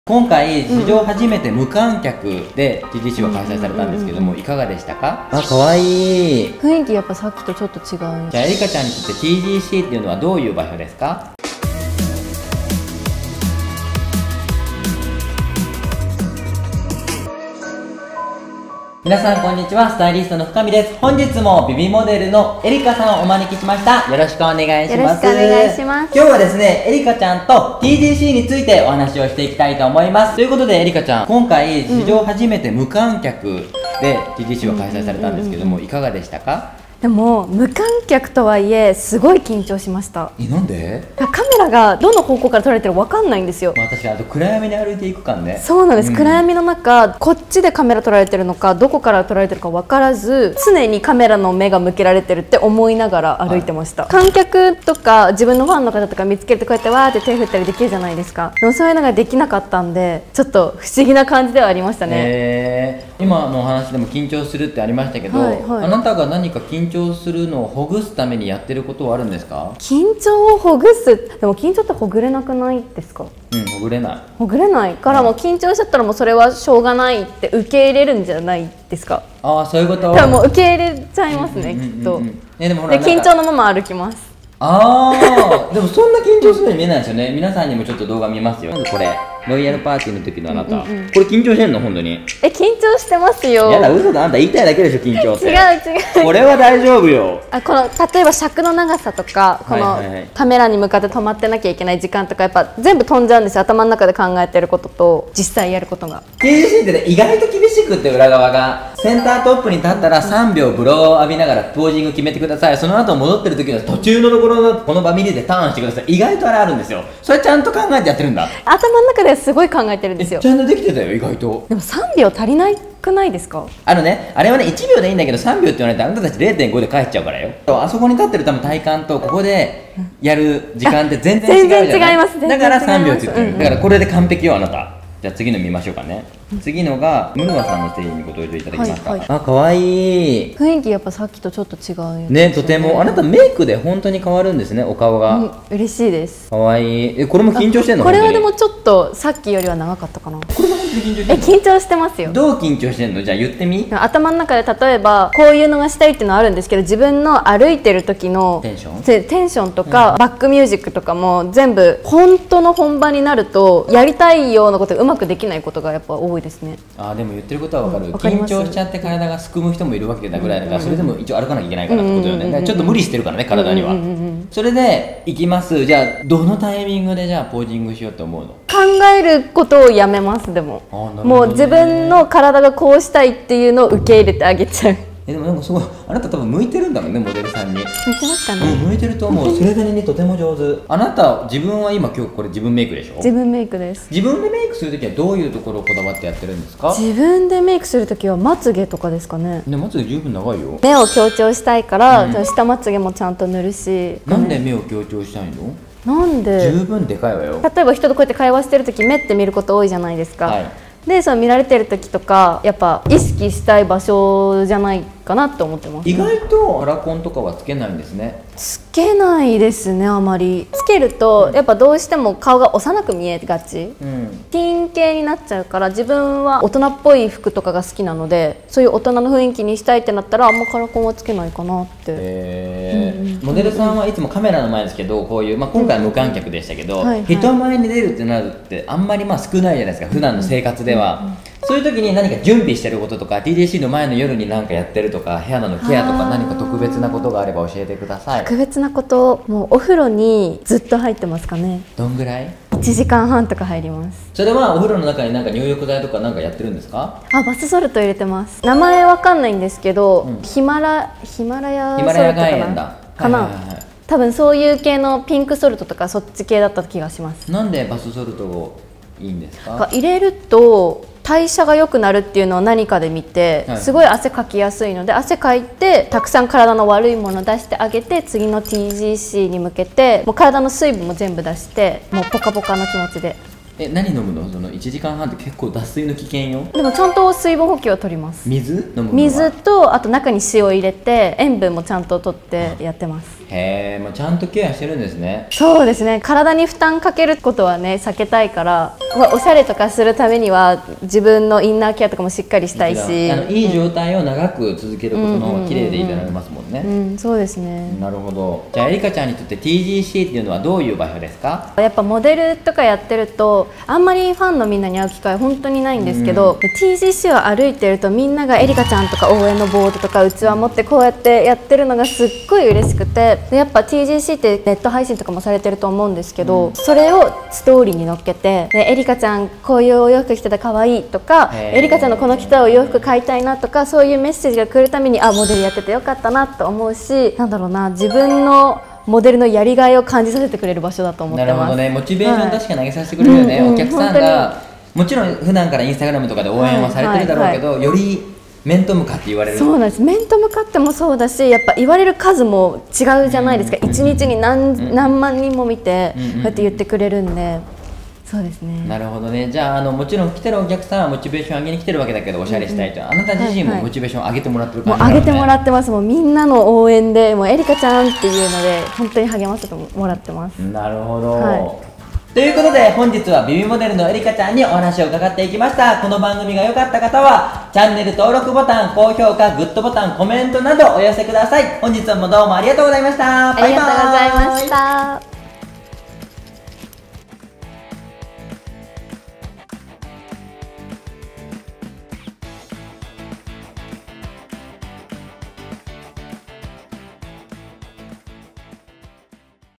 【初告白】ViViモデル・愛花 TGCへの思いを語る
ViViモデル・愛花がTGCの裏側やTGCへの思いをトーク！
TGC前にしている美容法も大公開✨

TOKYO GIRLS COLLECTION ／東京ガールズコレクション

🎤 MC
倉田乃彩